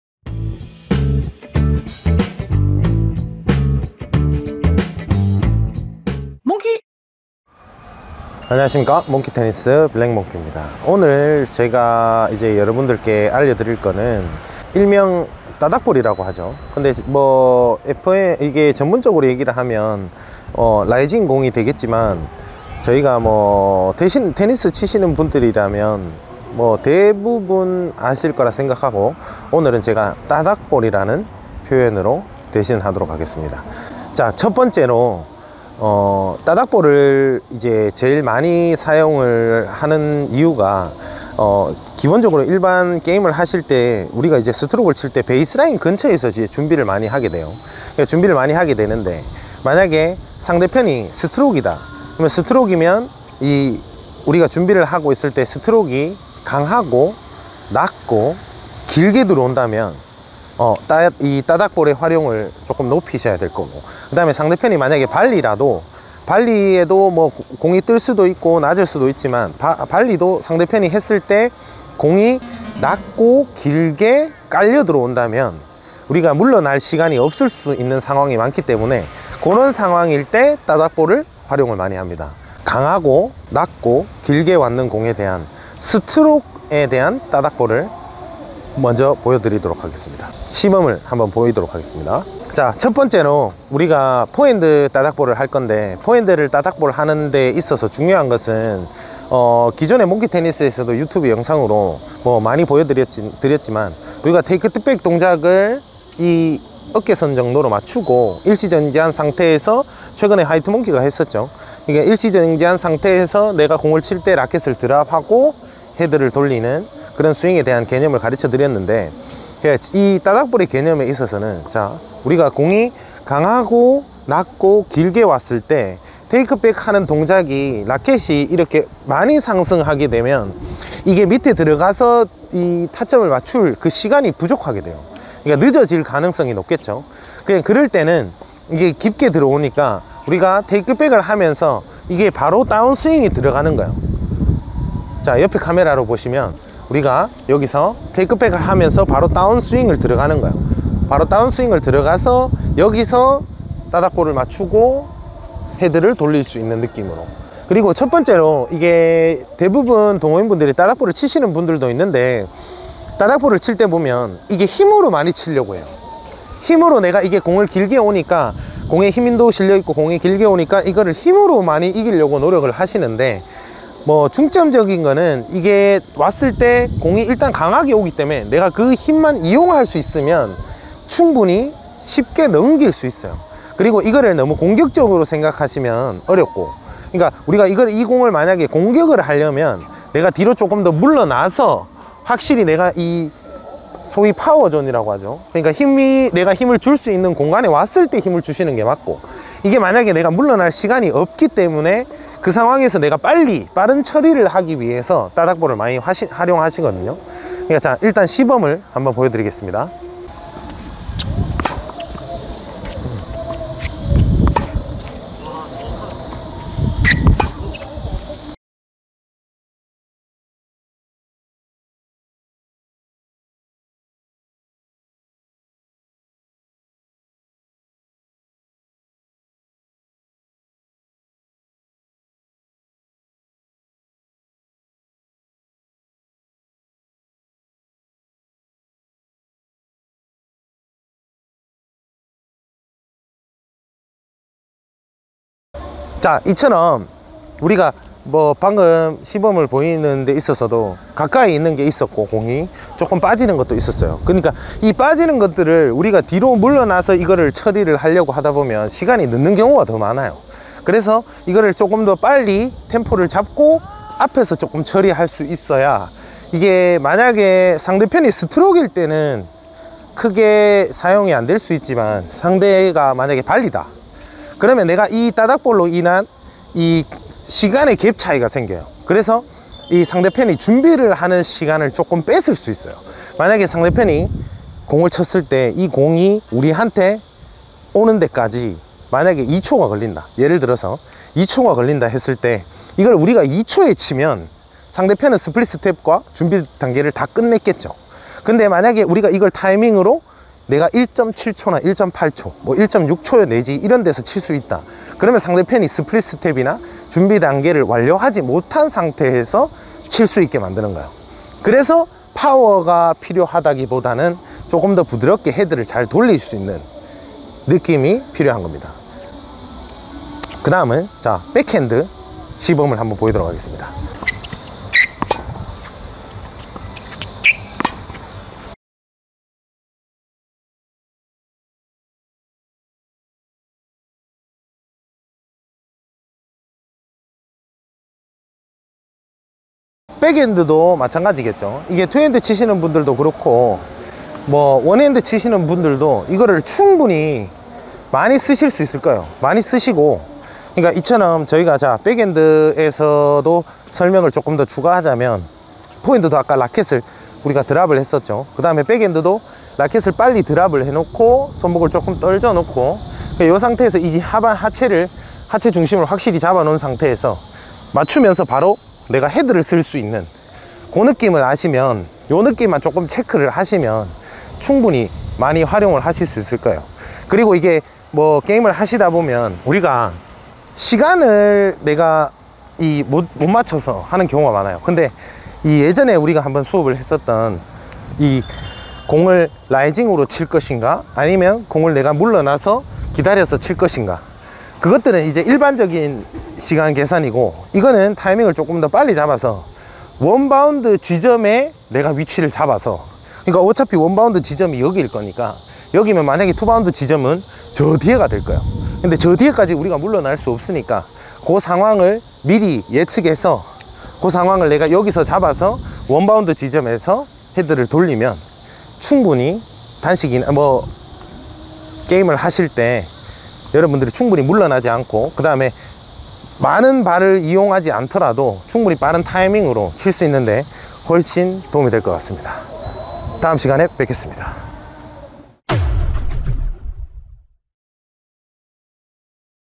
따닥볼은 이렇게!!블랙몽키가 알려주는 베이스라인 따닥볼 꿀팁!
오늘은 라이징볼에 대한 강의입니다.

감사합니다^^;